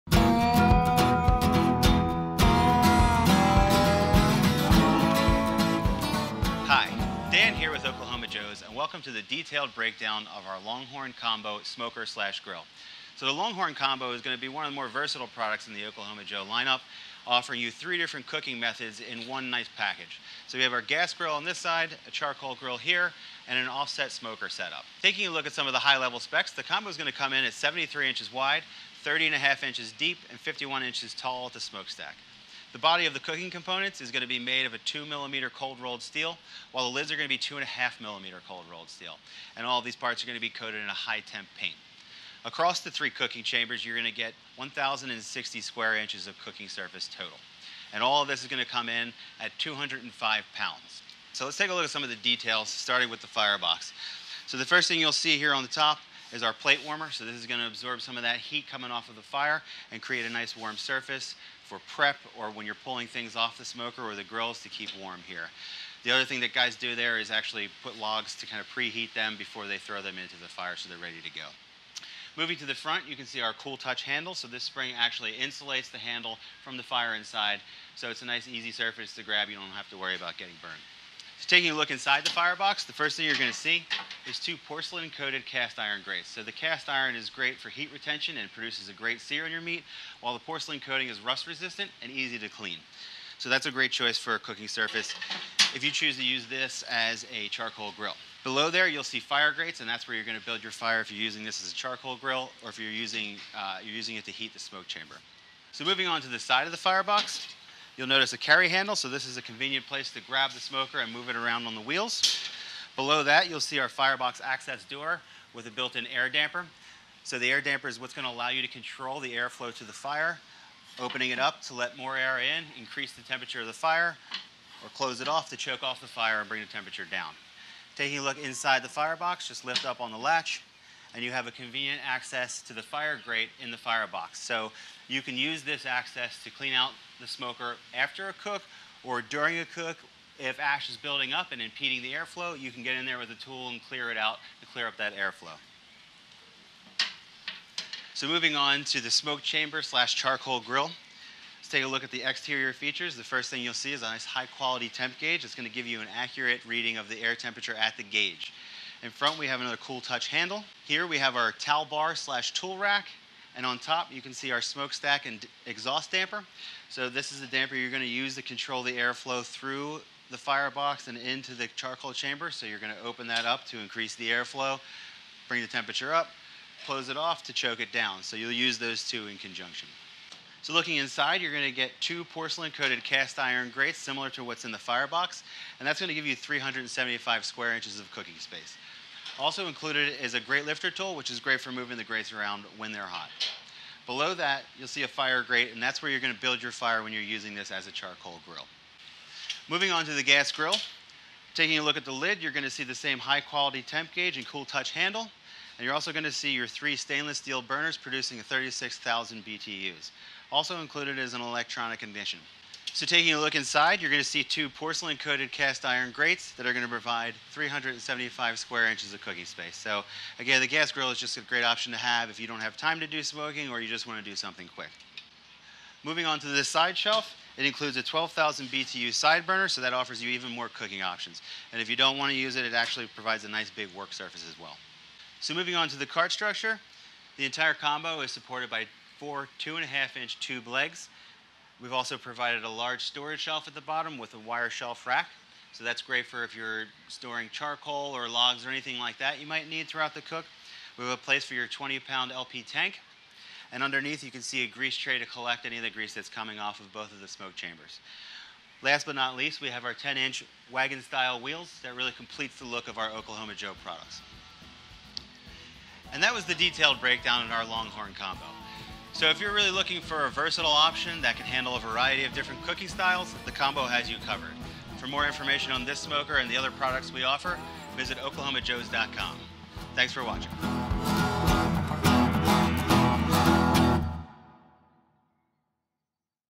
Longhorn Combo Charcoal and Gas Smoker - Product Walkthrough | Oklahoma Joe's
The Oklahoma Joe's Longhorn Combo Charcoal and Gas Smoker is perfect for a day of smoking and grilling. This three-in-one combo grill and smoker combines the convenience of an electronic ignition with a rugged offset firebox giving you BBQ just the way you want it. Product Manager Dan Corso provides a detailed overview.

With two separate cooking chambers, one for smoking and one for gas grilling, plus a sideburner for optional use, the versatility allowed by the Longhorn Combo lets you choose your preferred cooking method for your meal.
Keep track of the inside temperature of both the smoker chamber and the gas grill chamber with lid-mounted temperature gauges.
Get a firm grip on your grill or smoker lid while protecting your hands and fingers from heat with the Cool Touch steel handles.

Model# 15202029